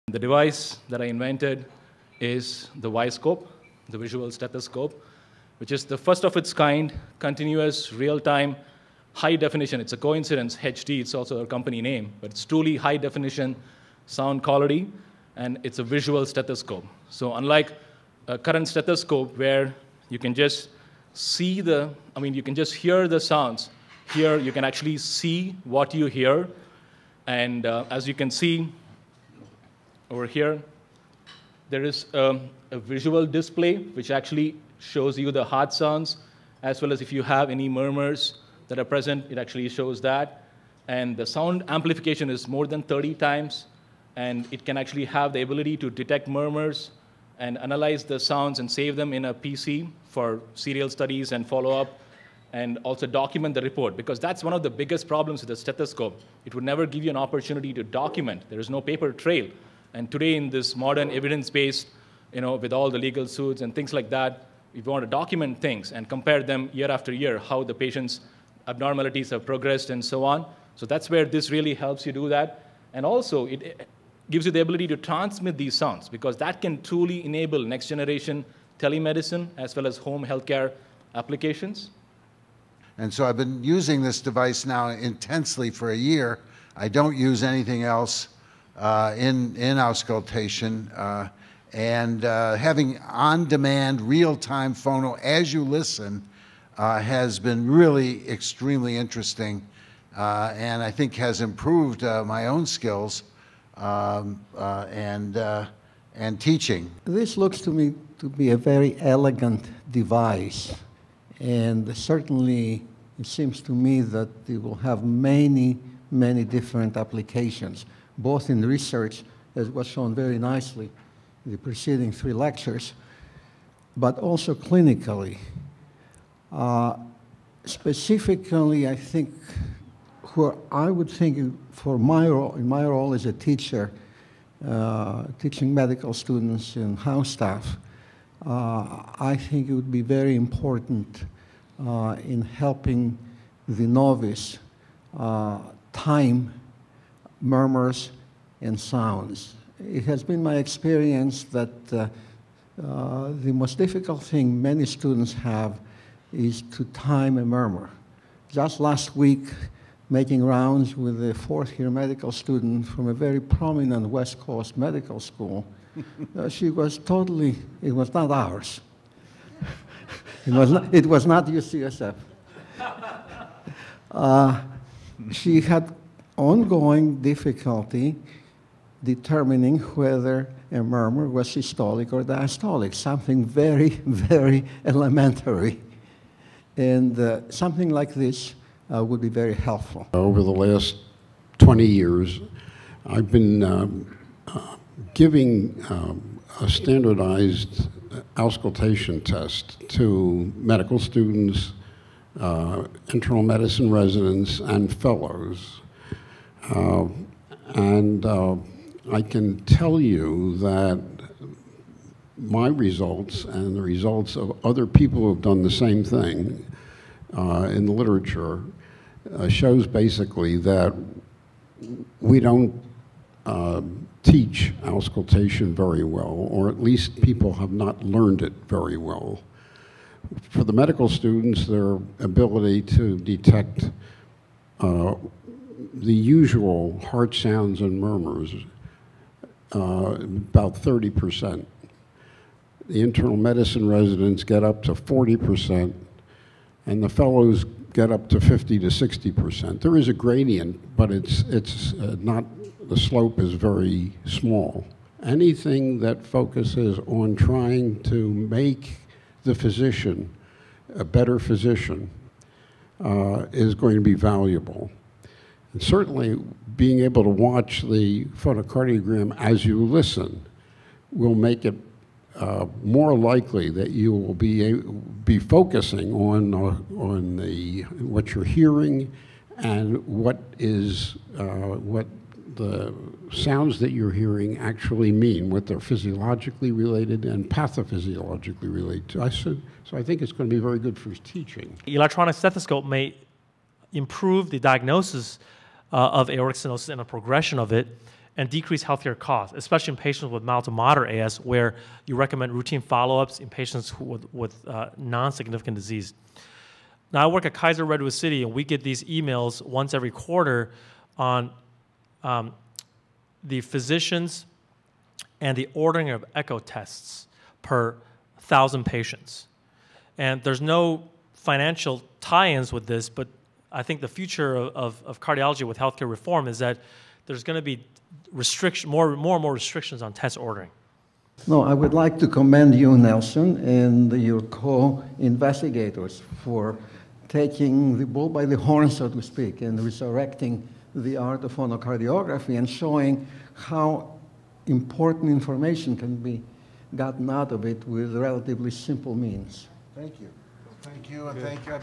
HD Medical Group ViScope medical endorsements
Leading physicians endorse the HD Medical Group ViScope Visual Stethoscope